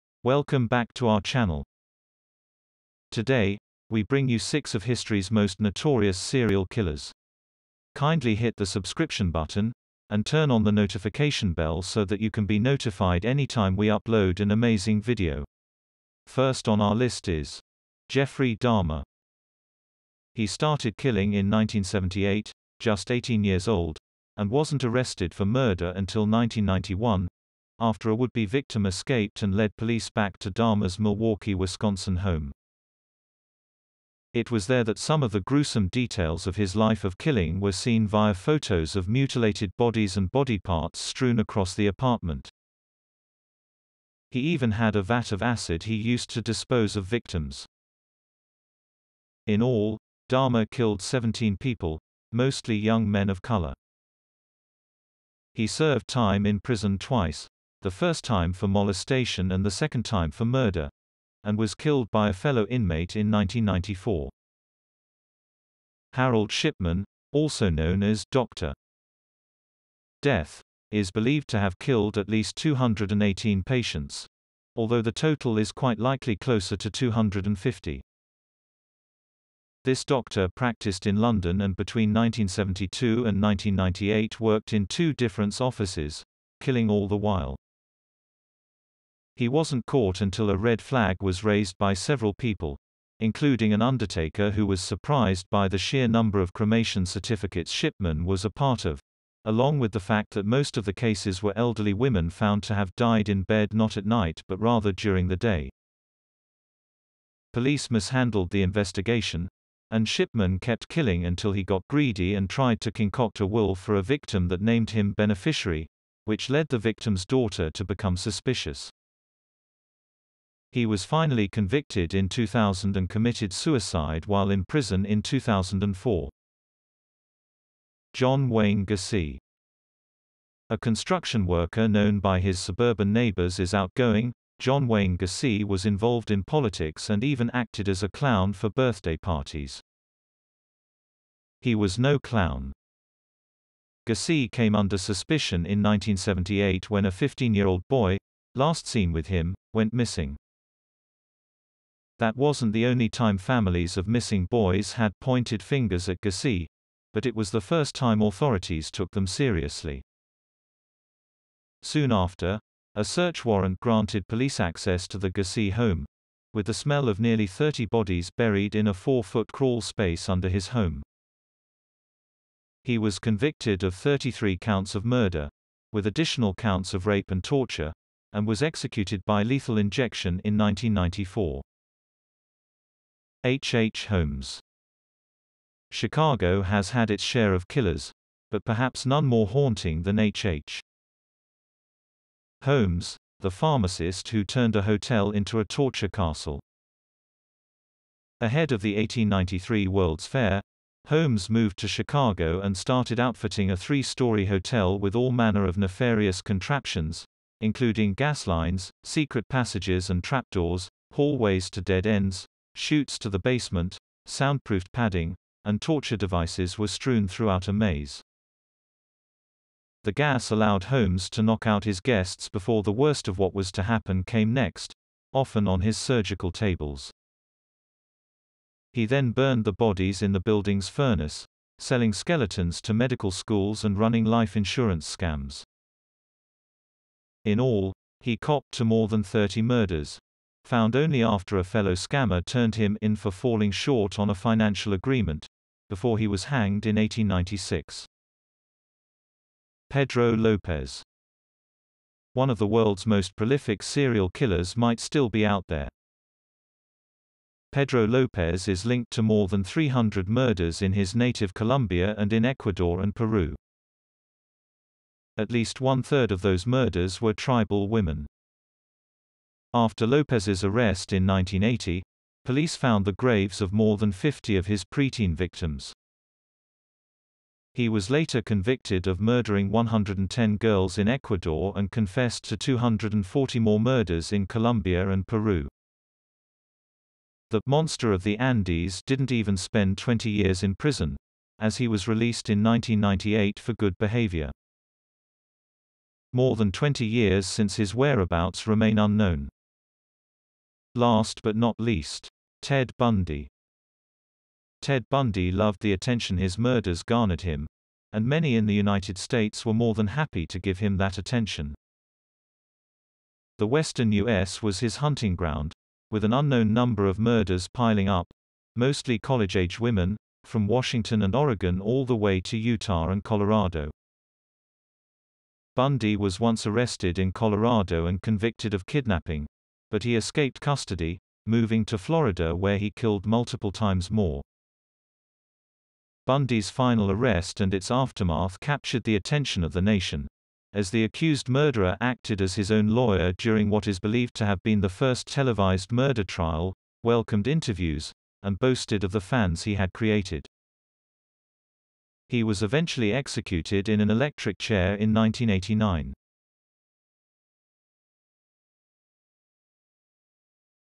TOP 6 HISTORY'S MOST NOTORIOUS SERIAL KILLERS
Today we bring you six of the world's most notorious serial killers of all time...